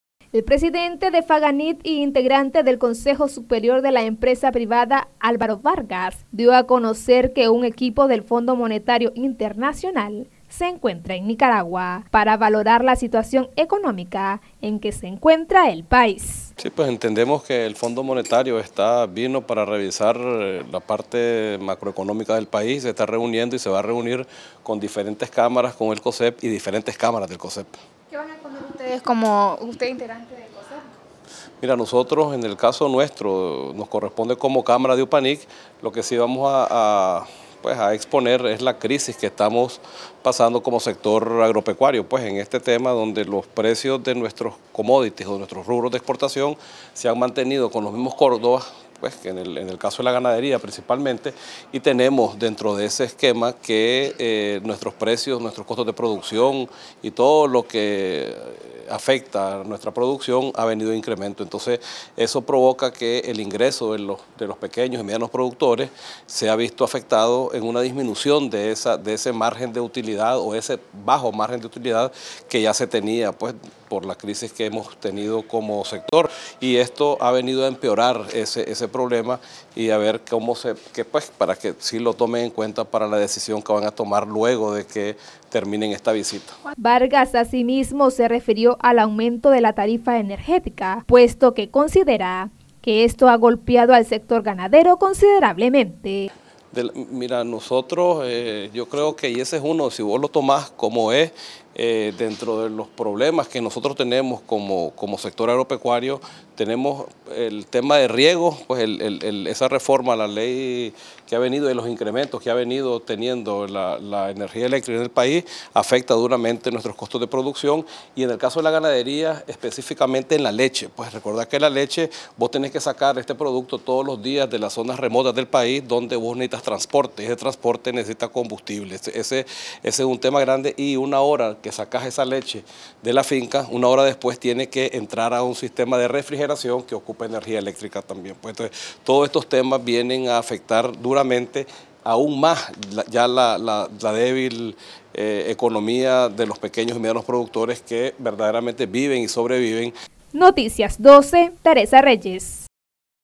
VARGAS: “FMI VALORARÁ LA SITUACIÓN ECONÓMICA DE LA PAÍS”
El presidente de FAGANIC Álvaro Vargas, afirma que un equipo del fondo monetario internacional se encuentra en el país para valorar la situación económica.